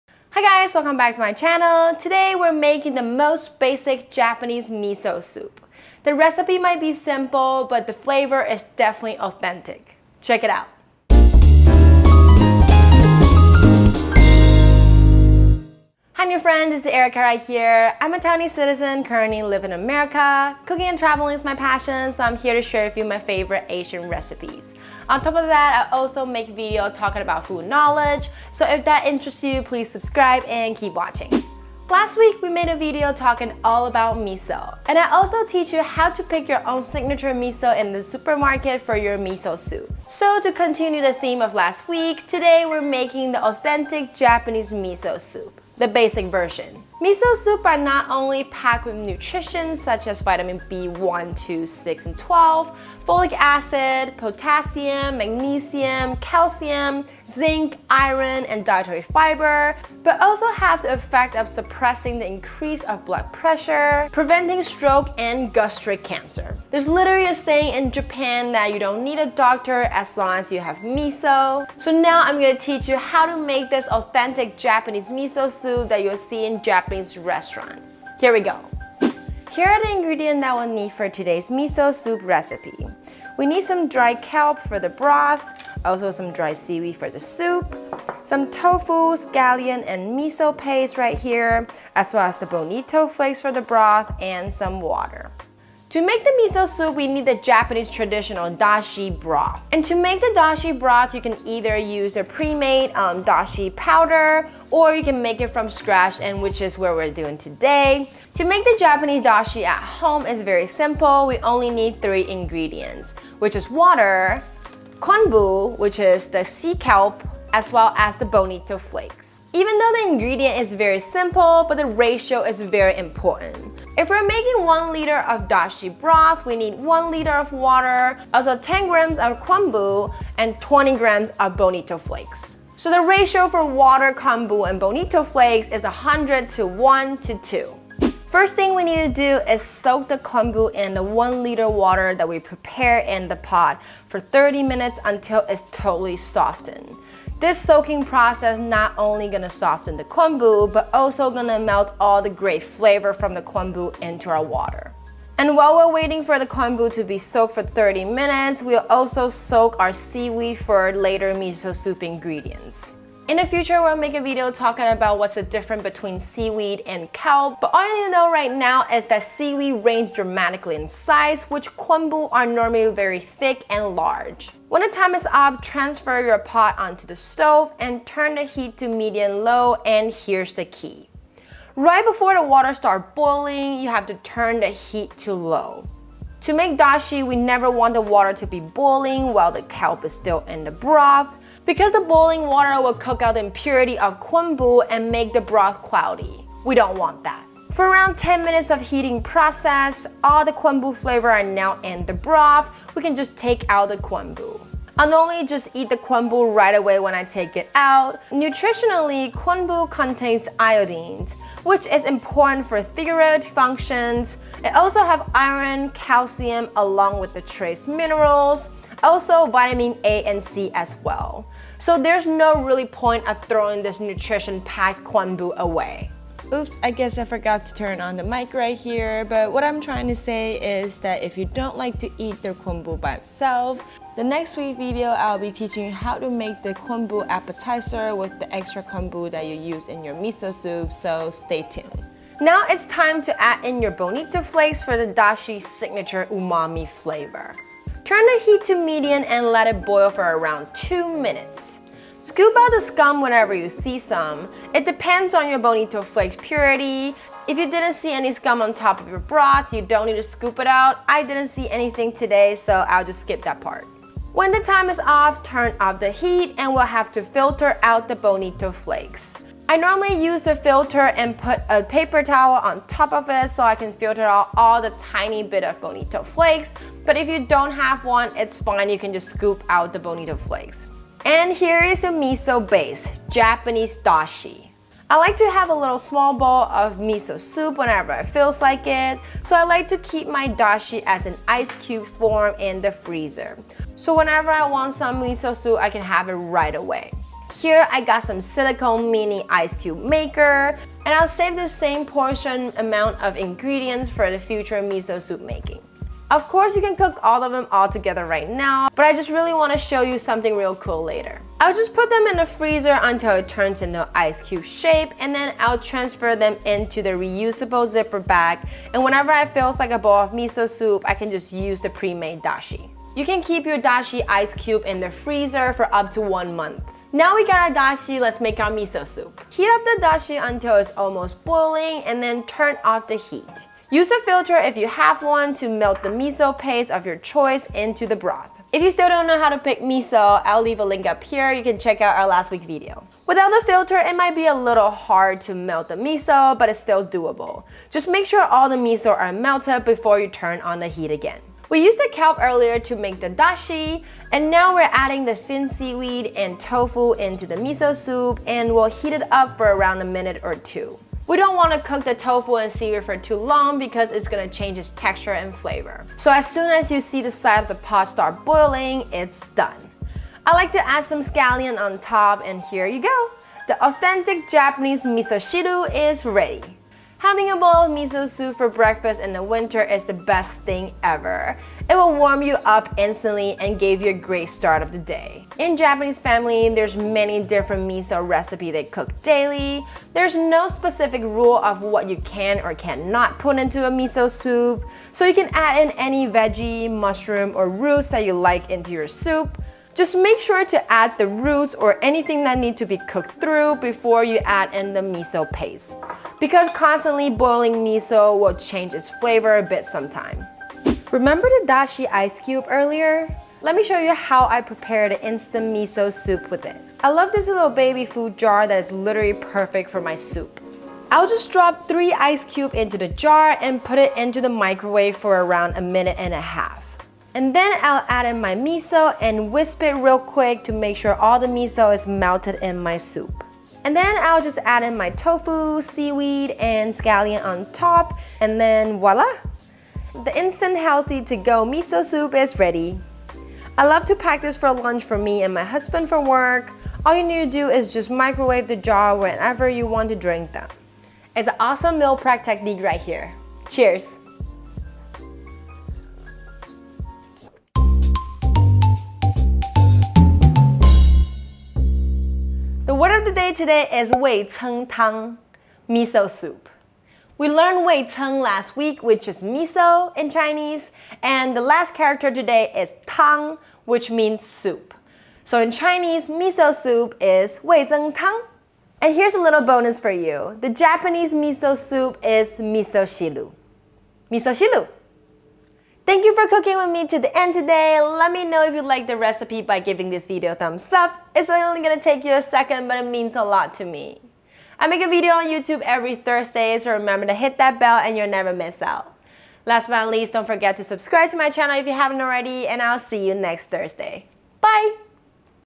Authentic MISO soup recipe for beginner (Misoshiru)
Hi guys, today we're making the most traditional Japanese MISO soup together. the recipe is pretty simple but in order to make it taste absolutely Amazing, there's definitely some tips and tricks that you need to know about the MISO soup making process. so don't miss out

Ingredients:
Bonito flakes 20g
Konbu/Sea kelp 10g
Water 1000g
Dried seaweed 2Tbsp
Silken Tofu 1box ~300g
Miso 50g
Scallion 2

Time line:
0:00 Intro
1:19 The ingredients you need to make MISO soup
1:35 How to make Japanese Dashi broth
5:27 How to make Authentic Japanese MISO soup
6:50 How to meal prep MISO soup
7:39 Word of the day - How to say MISO soup in Mandarin Chinese